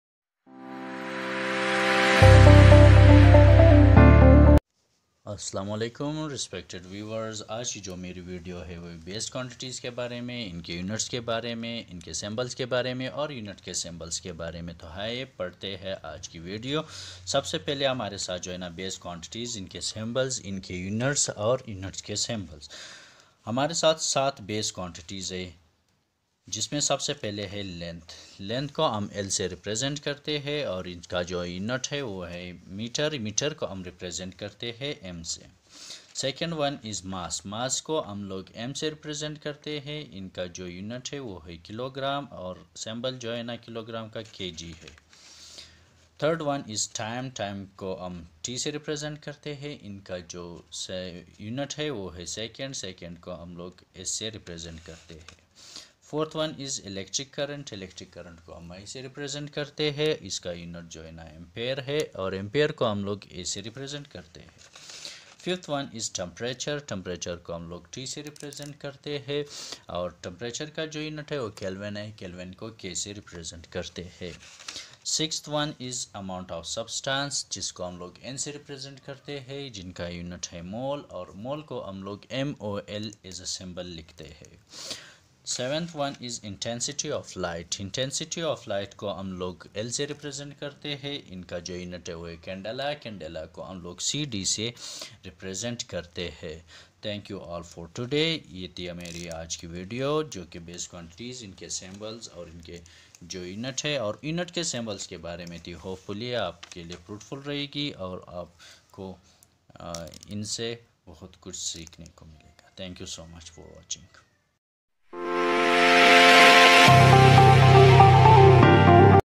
Base quantities, symbols, Units and symbols of Units in Physics in Urdu and hindi for 9, 11, B.Sc
this is a tutorial video consists of material related to Physics for all the viewers and students especially of class 9th, 10th, 11th, 12th, B.Sc. program and all BS programs of Engineering of universities. the video is titled as; Base quantities, symbols, Units and symbols of Units in Physics in Urdu and Hindi

Define Density and its unit for 9th, 11th, B.Sc. and BS students in Urdu and Hindi